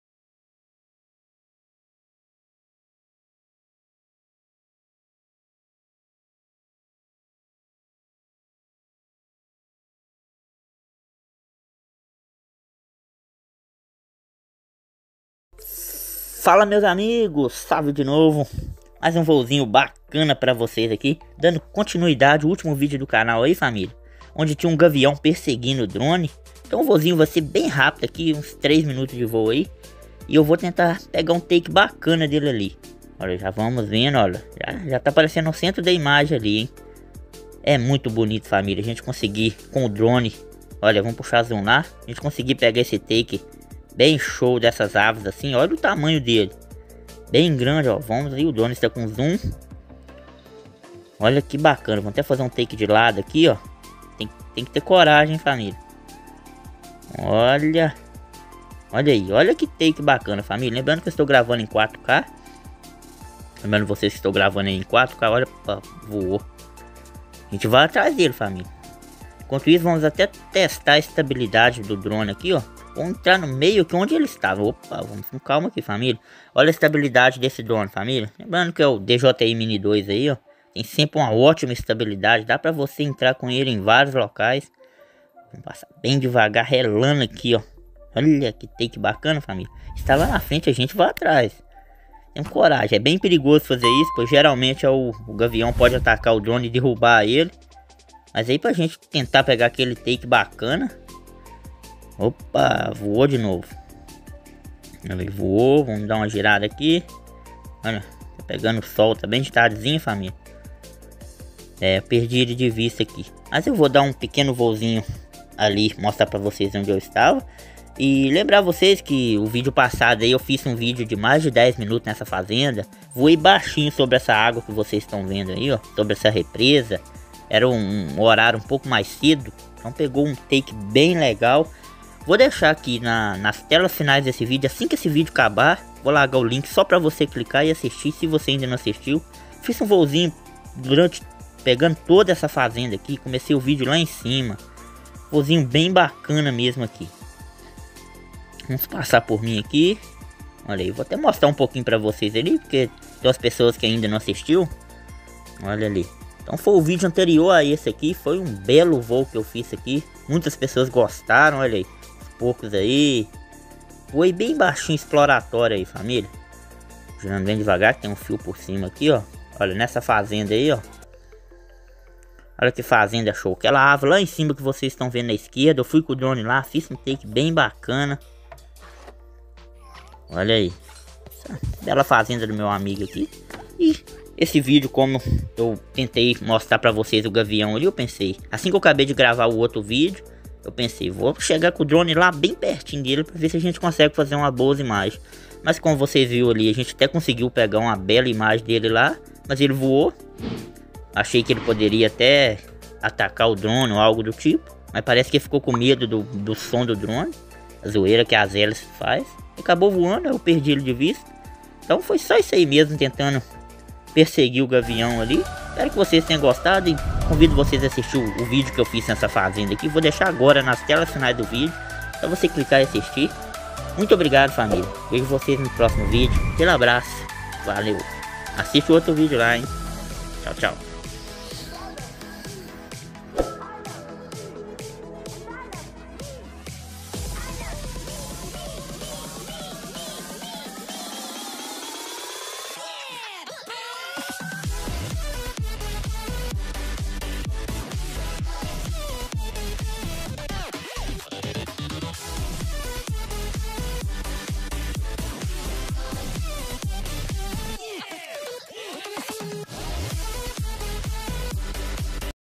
DRONE PERSEGUE GAVIÃO 🦅😱😱 teria coragem?
LONG RANGE DE 4 QUILÔMETROS COM O DRONE
TESTE DE DISTÂNCIA 👇👇

@juliobyte hobby

@multdrone hobby

@roni maximus hobbies

@câmera drone hobby

@viagem em fpv